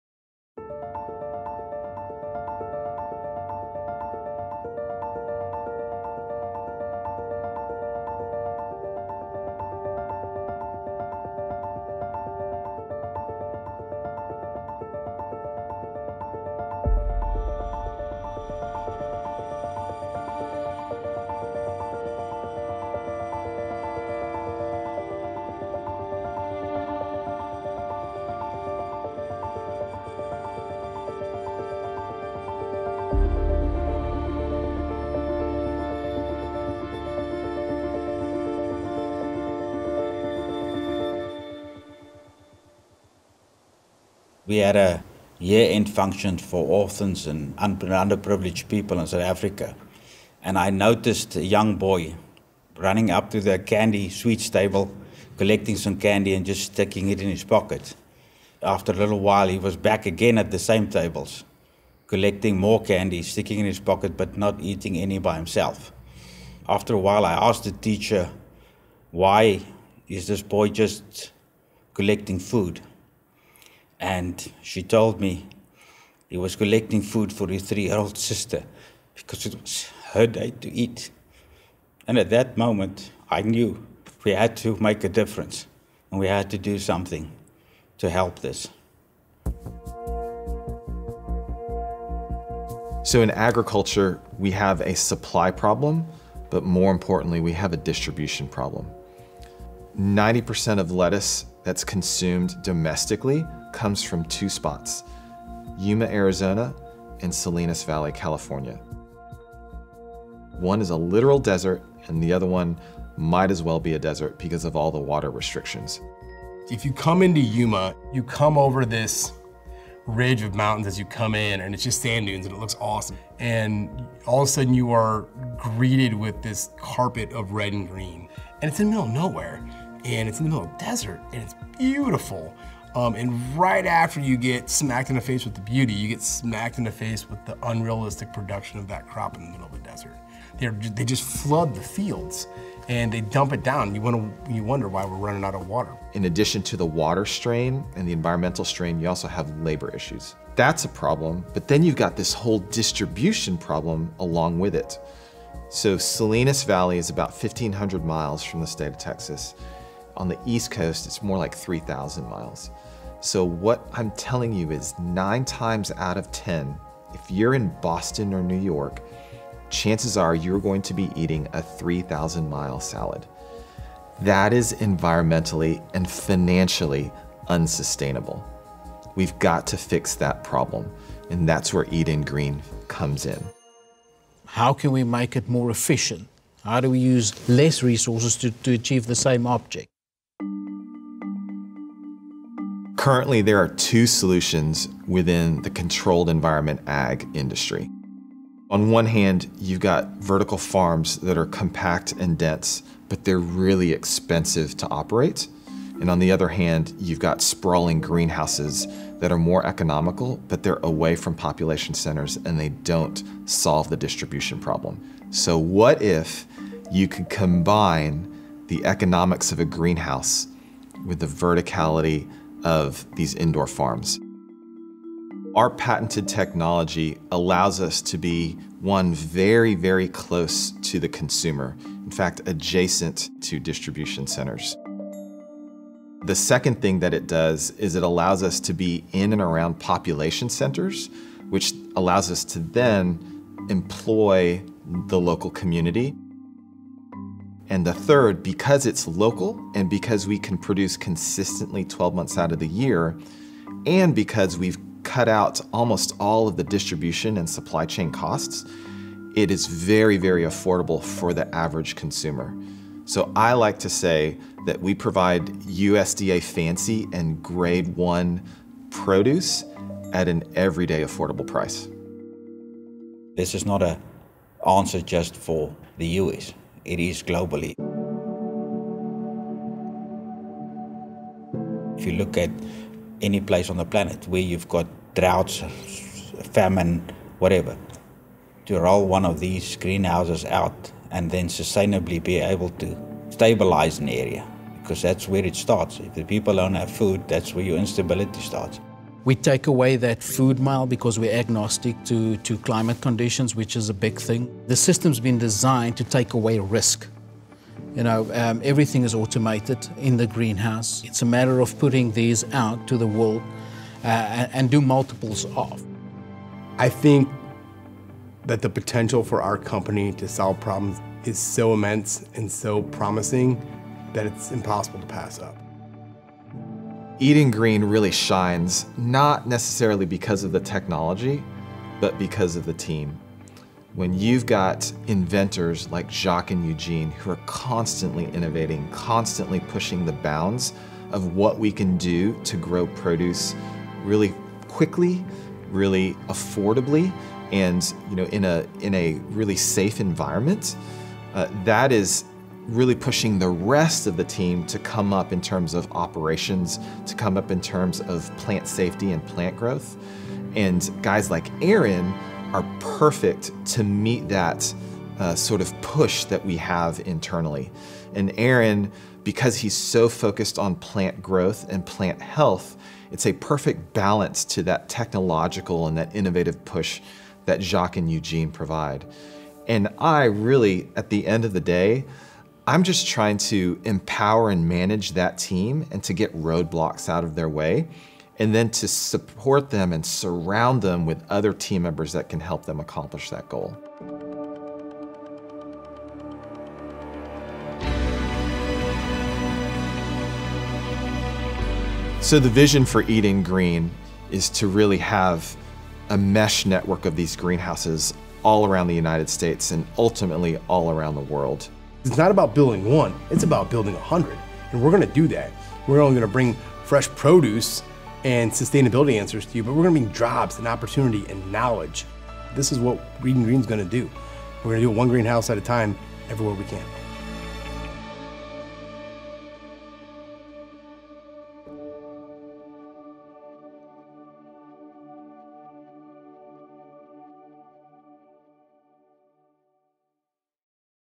Harvest to Home - Eden Green Technology's Mission in Vertical Farming
Eden Green is revolutionizing commercial agriculture one vertical greenhouse at a time. Our cutting edge hydroponic systems achieved what many thought wasn't possible — year-round harvests of locally grown produce people love to eat at prices shoppers can easily afford.

Learn more from our executive leadership team about how we're fixing the distribution, economic and labor problems related to the food supply chain in the US.